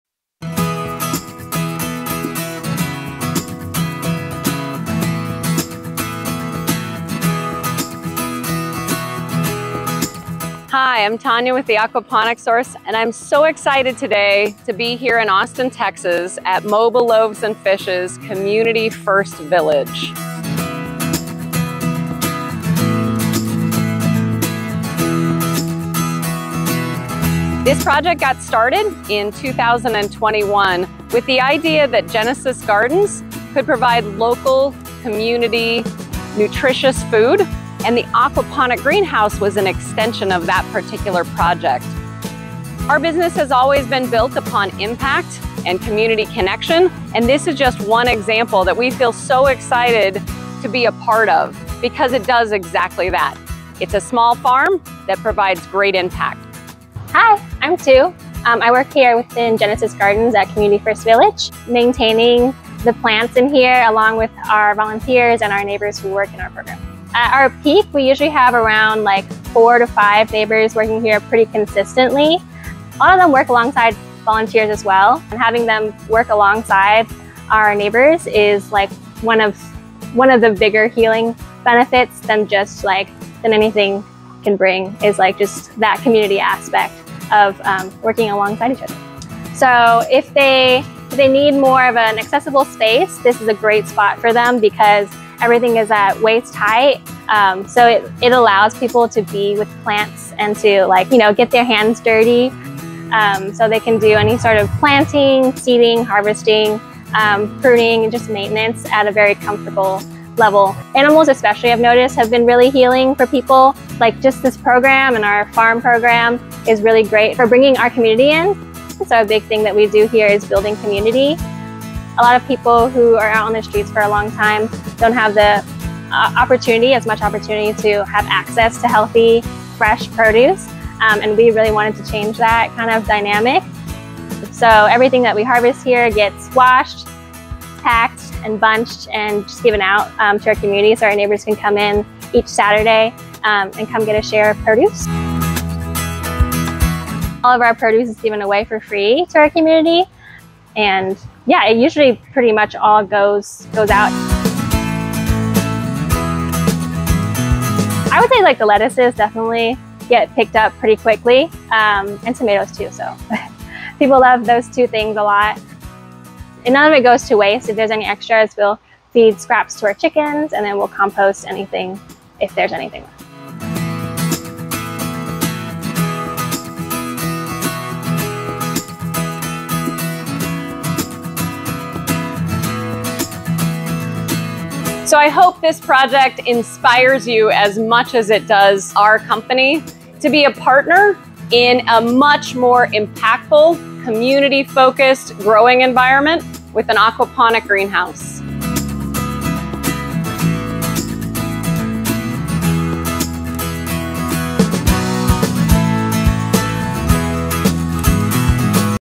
Inside the Aquaponic Greenhouse at Community First! Village | Sustainable Farming in Austin, TX
Community First! Village is a 51-acre community that provides affordable, permanent housing for hundreds men and women that previously experienced chronic homelessness in Austin, TX. The Village contacted The Aquaponic Source in 2020 for a custom-designed aquaponic farm to be built in their village to provide onsite food, education and job skill development for their neighbors.

On the Farm Training Is Back!

The Full Flourish Farm Online Aquaponic Class is available now!